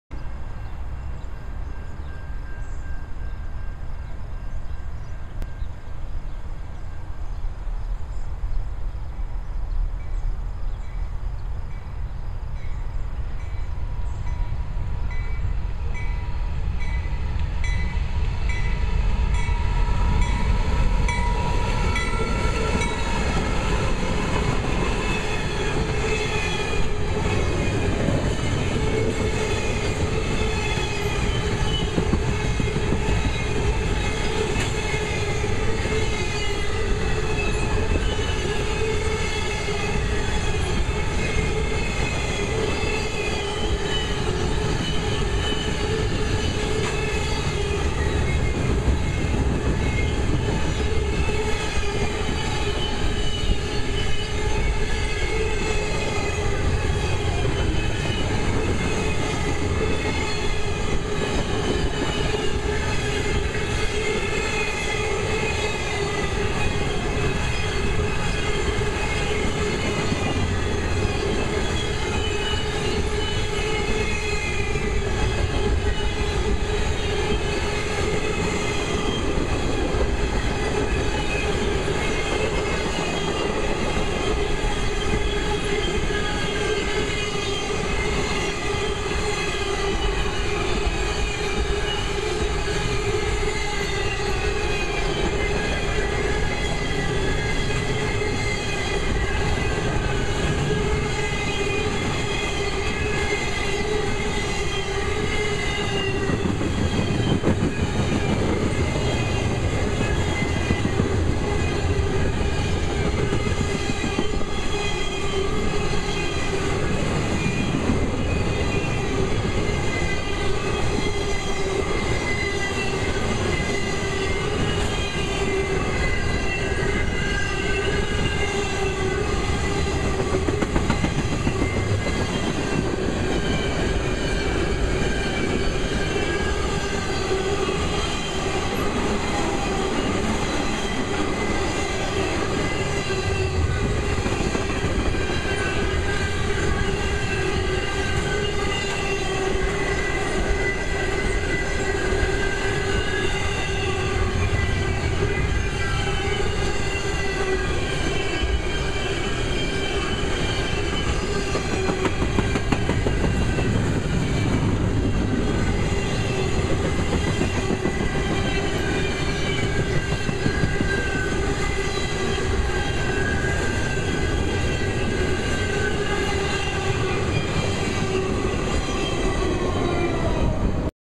CN 2515- C44-9WL Leads Stack Train, 6/4/2015
CN 2515 and 2408 (C40-8M) pull a southbound stack on the Leighton sub in Plainfield, IL